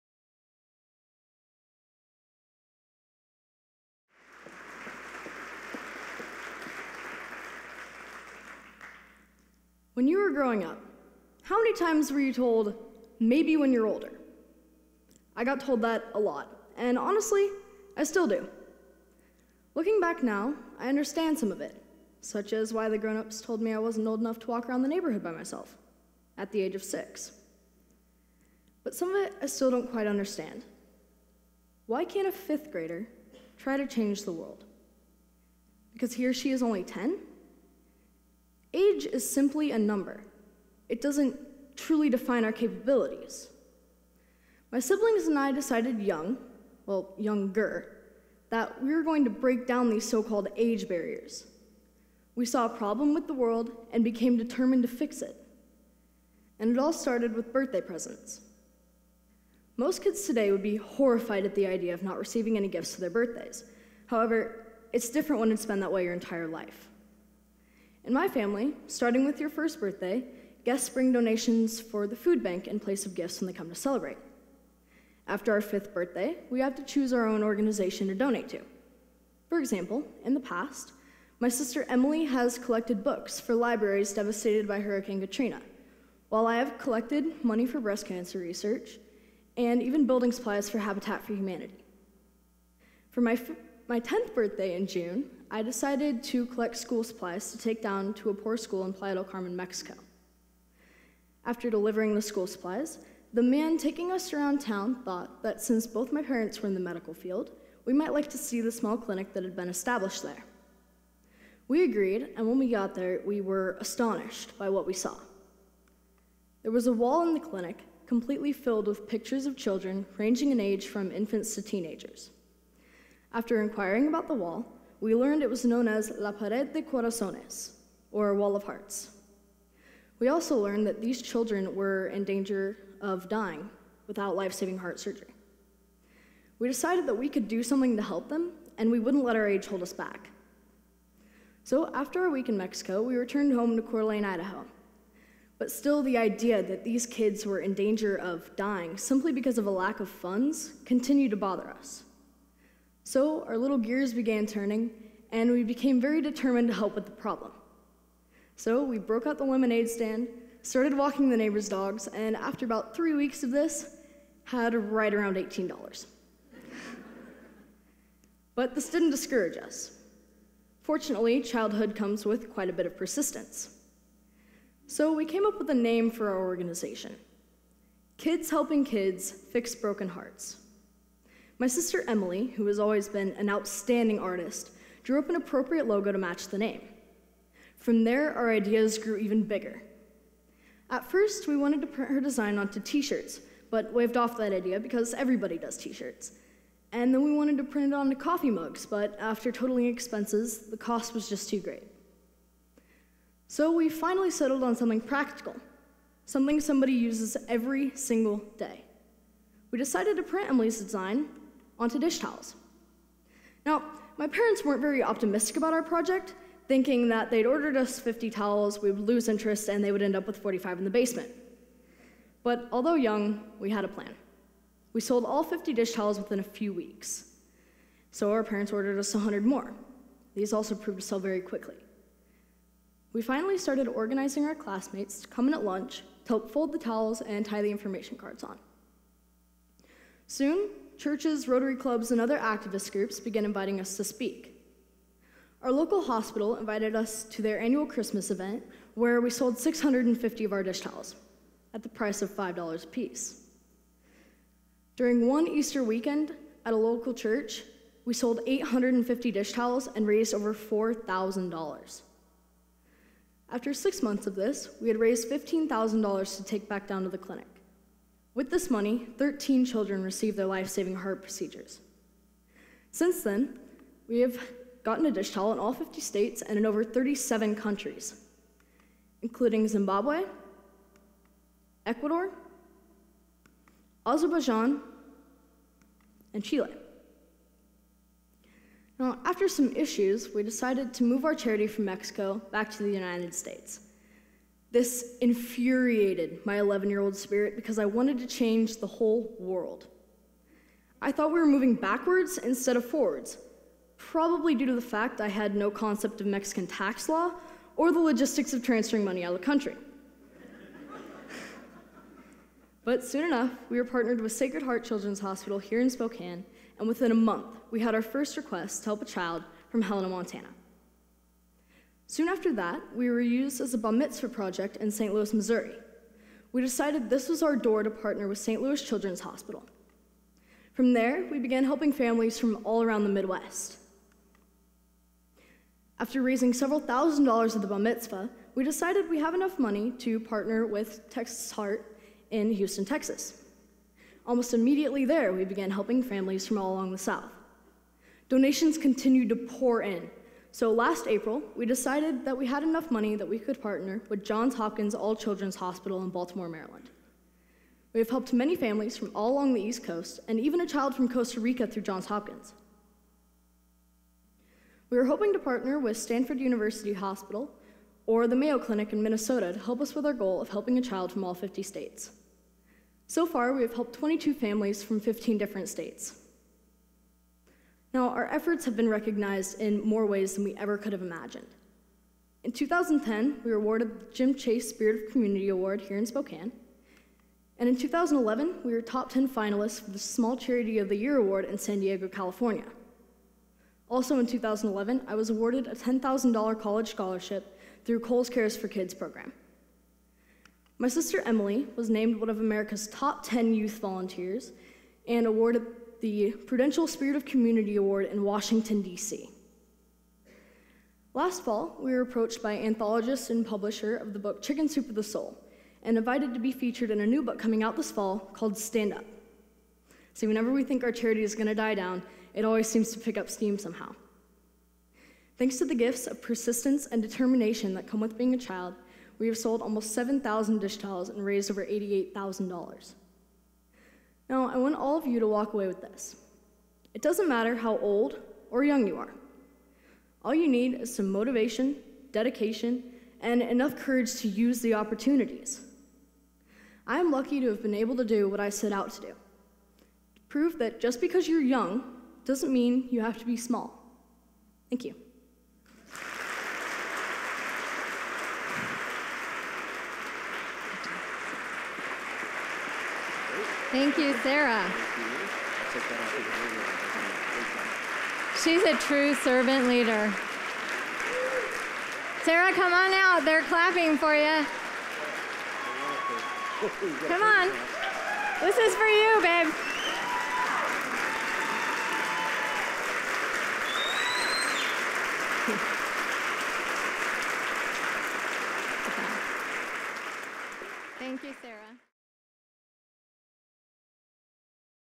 It started with an idea and a dish towel: Sarah Kladar at TEDxSpokane
A 14-year-old freshman, Sarah created a non-profit organization with her siblings four years ago to help families of children needing heart surgery far from their hometowns. It started with "an idea and a dish towel." Sarah will share her story of helping families with children facing heart surgery. This powerful story shows age is not an obstacle when achieving powerful results for others. Sarah and her siblings have raised over $90,000 and built partnerships with hospitals in seven states in helping others.